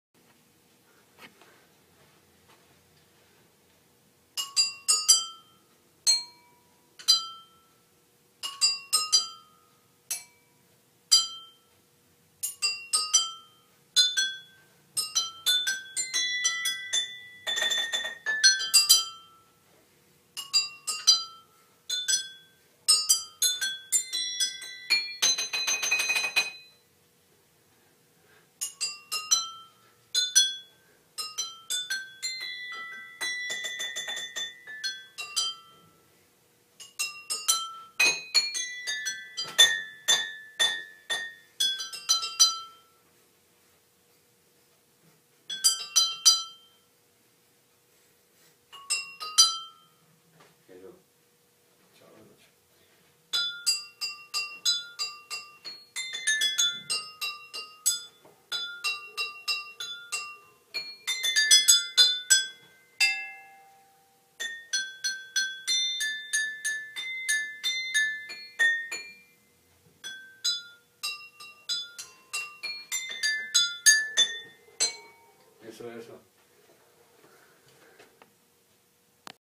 Aguófono
Dos temas muy populares interpretados en este instrumento musical casero.